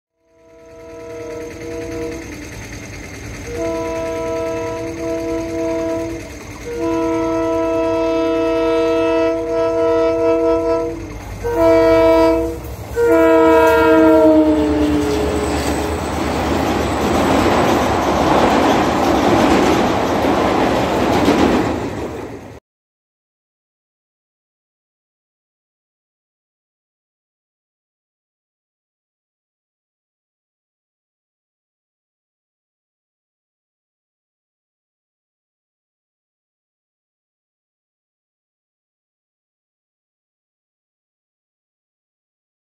15811 DHUBRI - GUWAHATI EXPRESS hauled by New Guwahati (NGC)'s WDP4D EMD || Abhinav LHB
15811 Dhubri - Guwahati Express hauled by customary 4500hp dual-cab EMD WDP4D 40607 of DLS New Guwahati (NGC), NFR; thrashes towards Guwahati, spotted in the outskirt of Guwahati.

I hope you will enjoy watching the video !